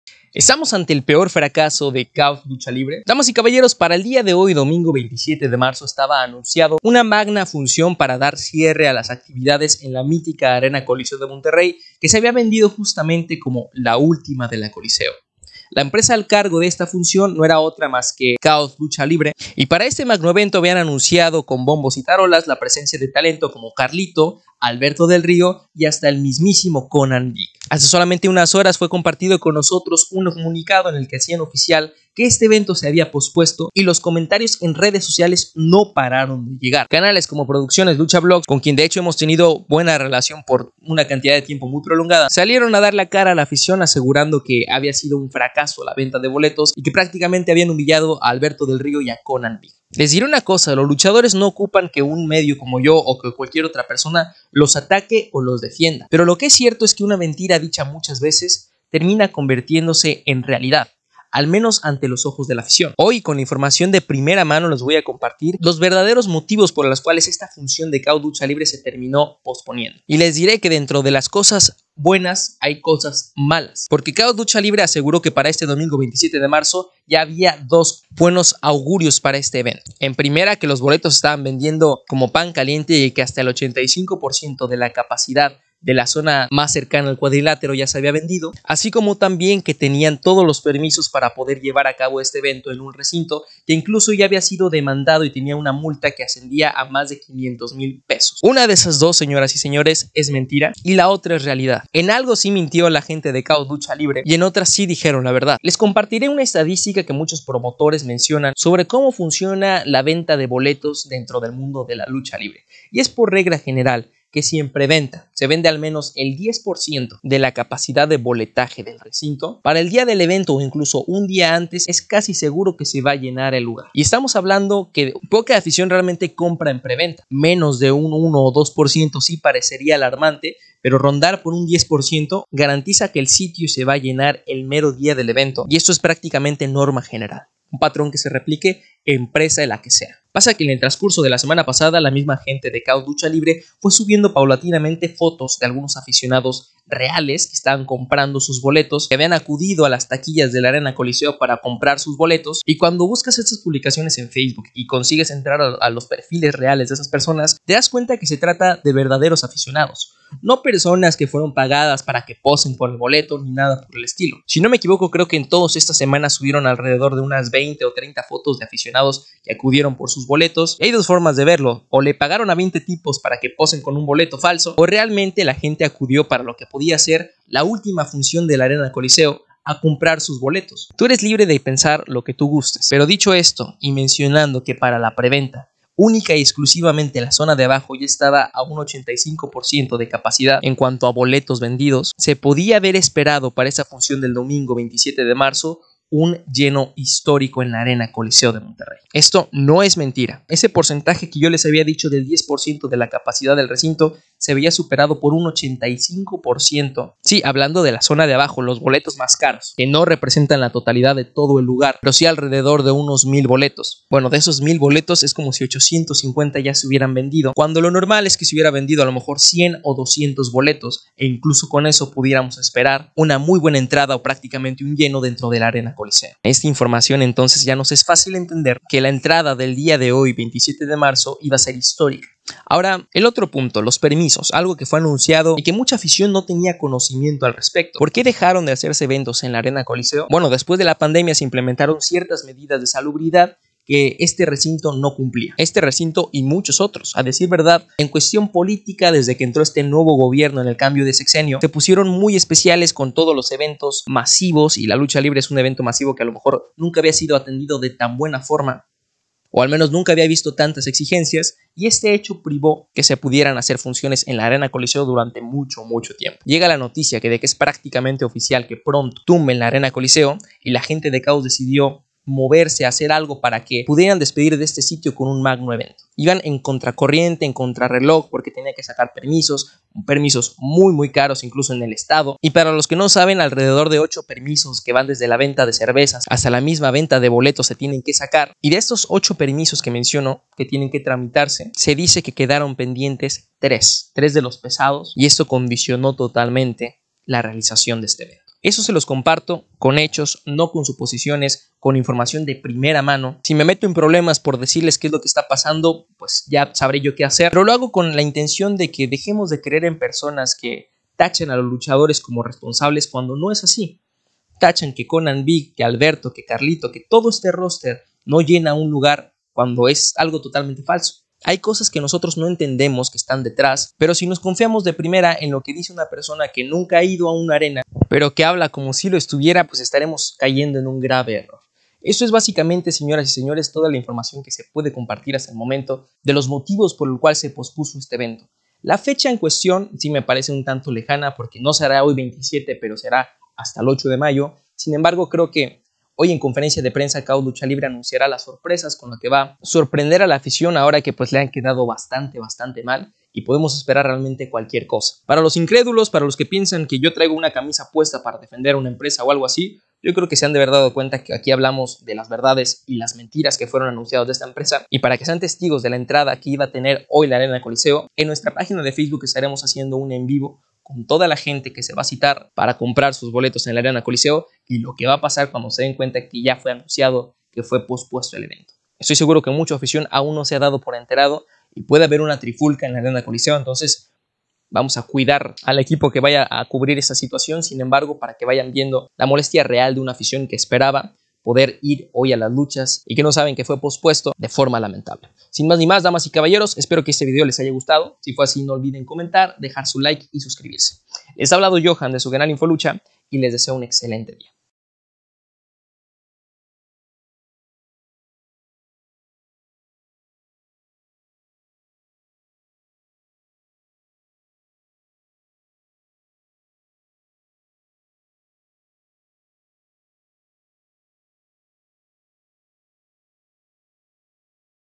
La VERDAD detrás del FRACASO de KAOZ Lucha Libre...
En este video hablamos sobre lo sucedido en la arena coliseo, luego del anuncio de la suspensión del

Si quieres ir aún más allá, dale a la campanita.